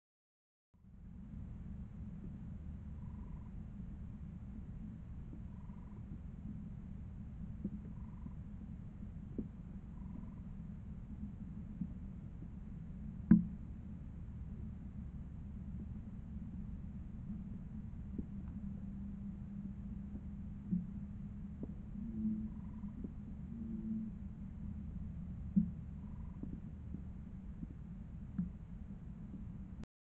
Kiitty Trap July 2nd 2023 11:15pm
Deployed Sardines, Nip and this is what we got.